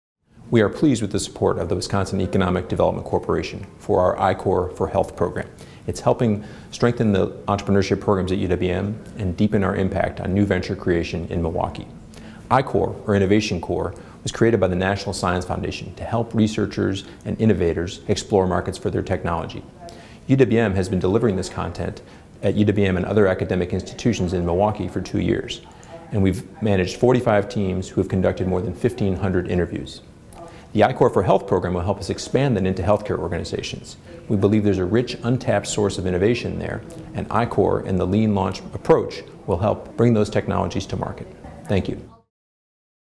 UWM receives state grant to support entrepreneurship
The University of Wisconsin Research Foundation was awarded a $33,000 grant from the Wisconsin Economic Development Corporation (WEDC) to teach healthcare professionals how to turn their ideas into startup businesses.

The I-Corps program in Milwaukee teaches students and academic researchers at five universities how to find out what customers want before spending a lot of money on developing an idea.